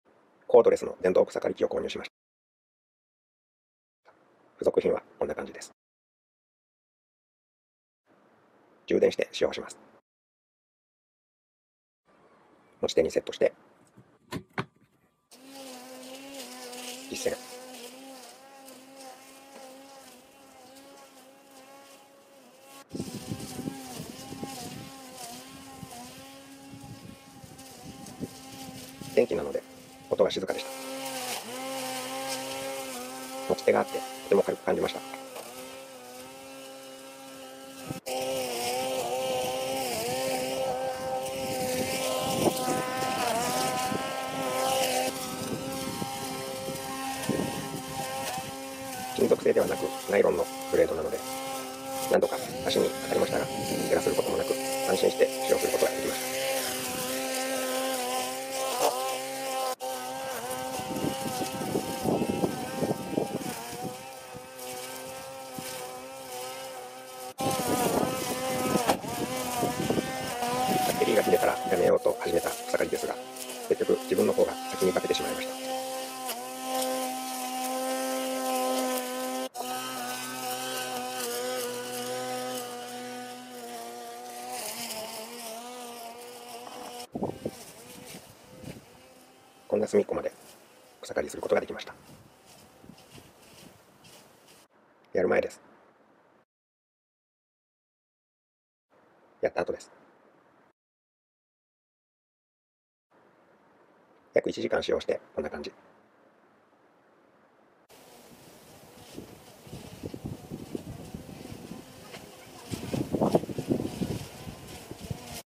Revisión de siega eléctrica inalámbrica
Usé una cortacésped por primera vez.

Incluso el tipo eléctrico tiene suficiente potencia.

Es la sensación de usar una aspiradora al aire libre.

Las cuchillas de nylon eran seguras de usar incluso para principiantes.

Cortar el césped a mano también es bueno, pero las cortacéspedes de césped son convenientes para una amplia área.